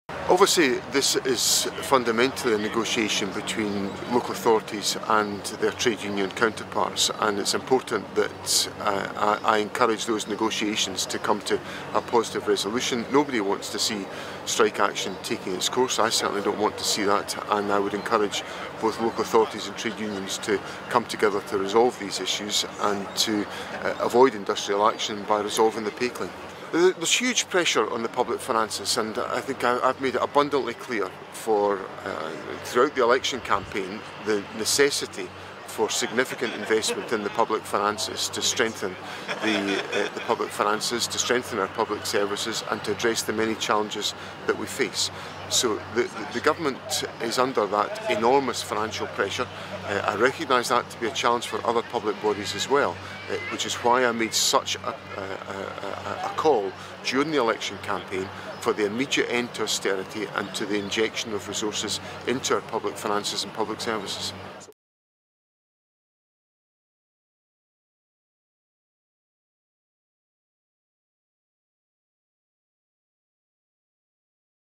Bin strikes: John Swinney refuses to get involved and blames councils
John Swinney refused to get involved in the ongoing bin strikes row as he tried to dodge responsibility for rubbish building up in Scotland's cities this summer. Unions are planning industrial action in the coming weeks, meaning a repeat of two years ago when garbage filled the streets in Edinburgh during the Festival.

Refuse workers at councils could be about to go on strike after failing to agree a pay deal with Cosla, with this affecting the majority of local authority regions. Calls have been made for the Scottish Government to step in and strike a deal, much like when Nicola Sturgeon did so in 2022.